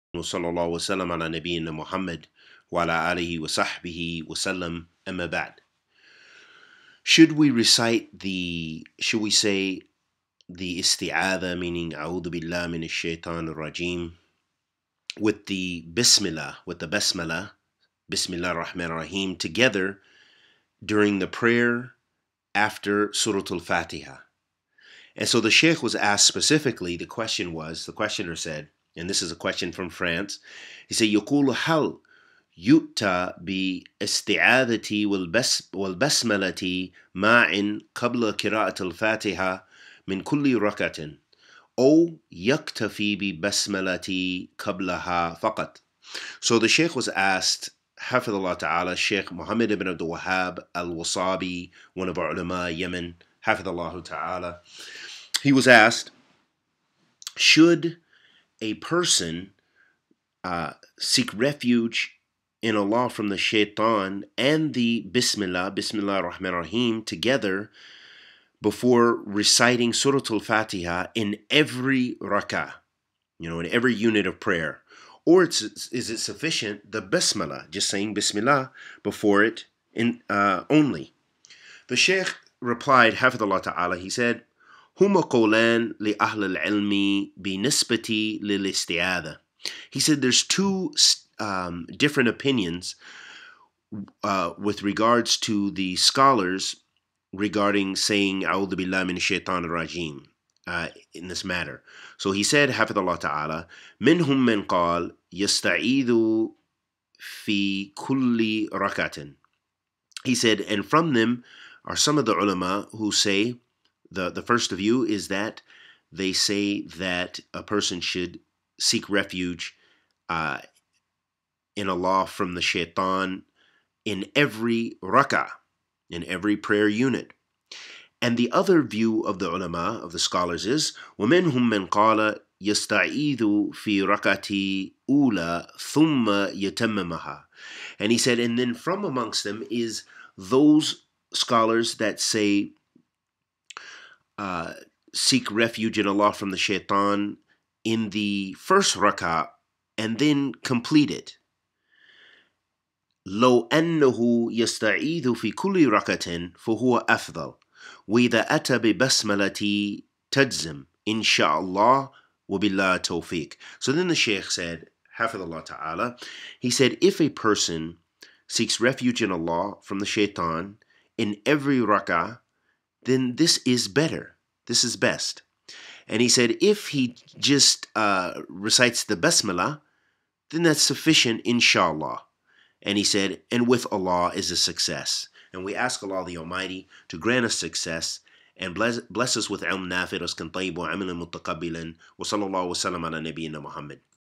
Should I seek Refuge In Allah From Shaitan In Every Rakaat?
Fatawa regarding salaat shaykh Muhammad Ibn Abdul Wahhab Al-Wasaabee